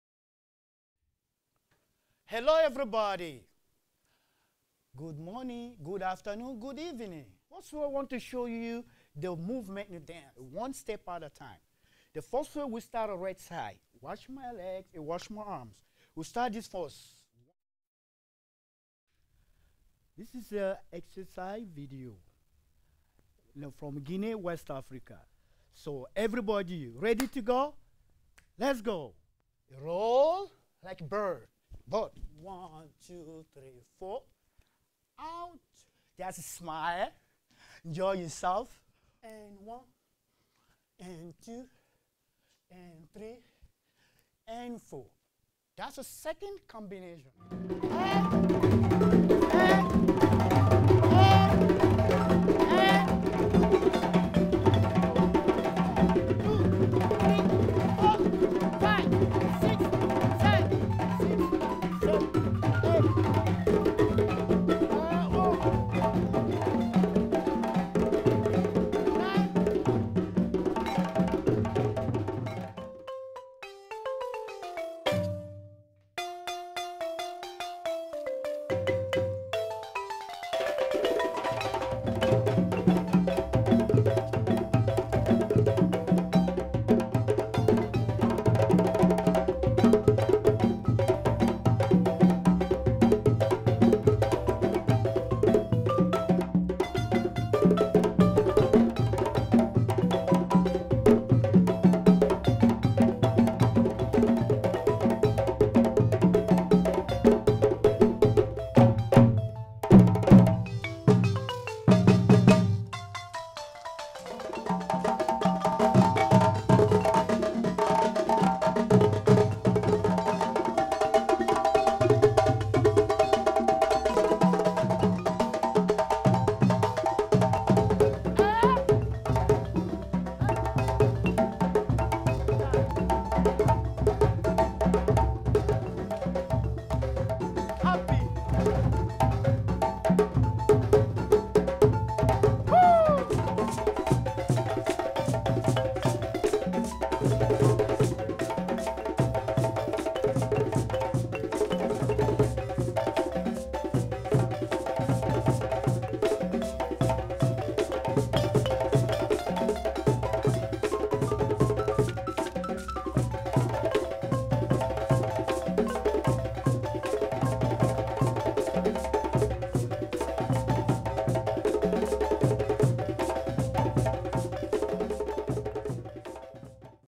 Abdoulaye Promo Vid
This is a promotional video for Abdoulaye Sylla's African dance instructional DVD that comes out this spring.